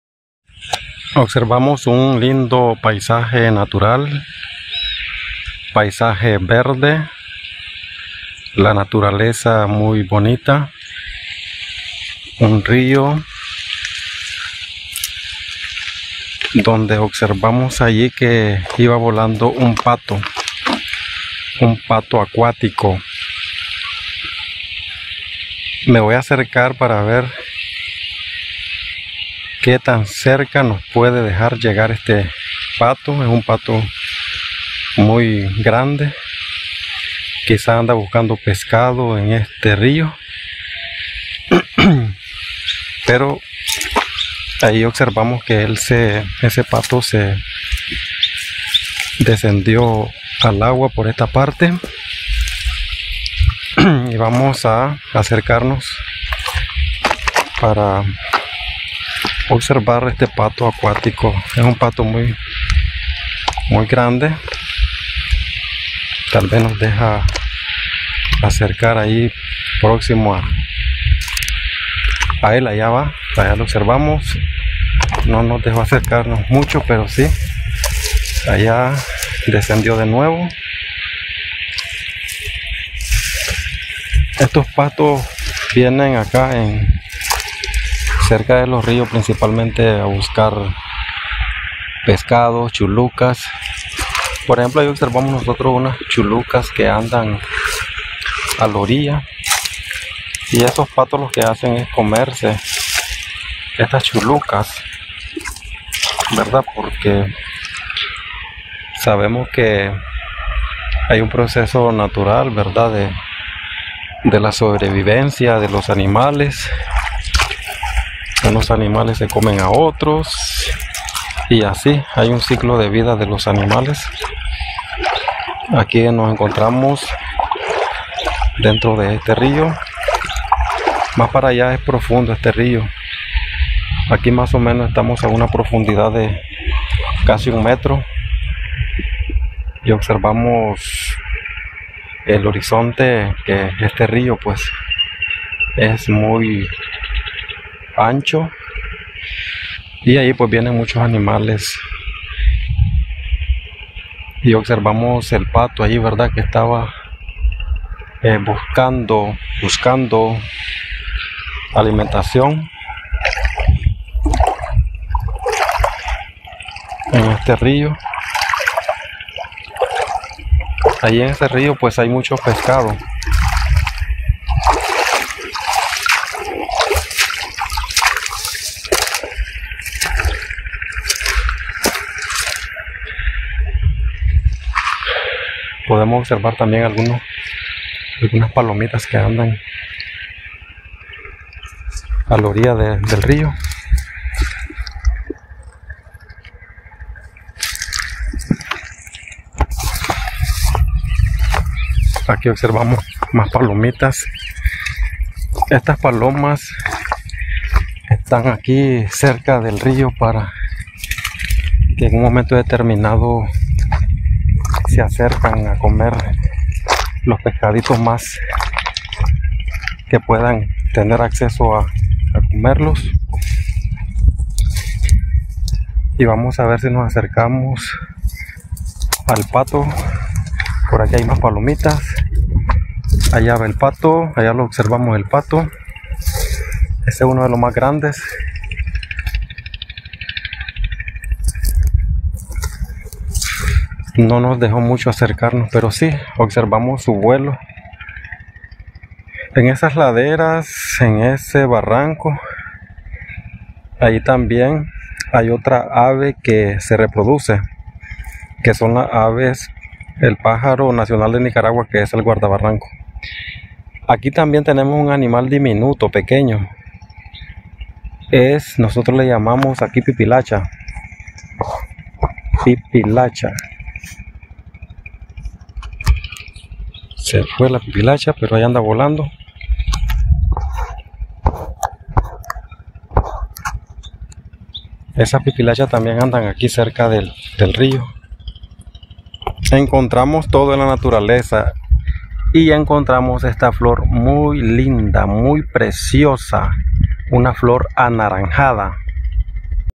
Buscando el Patito acuático. en el Río, lindo paisaje natural
Fuente de agua, animales acuáticos y los patitos en el rio